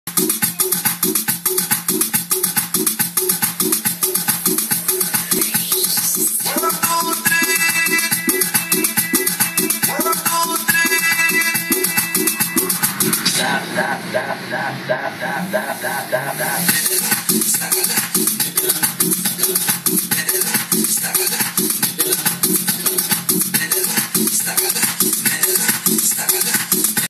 Making a mashup up punta remix DJVPO
working on this punta edit i have long way o go for making it ready for use,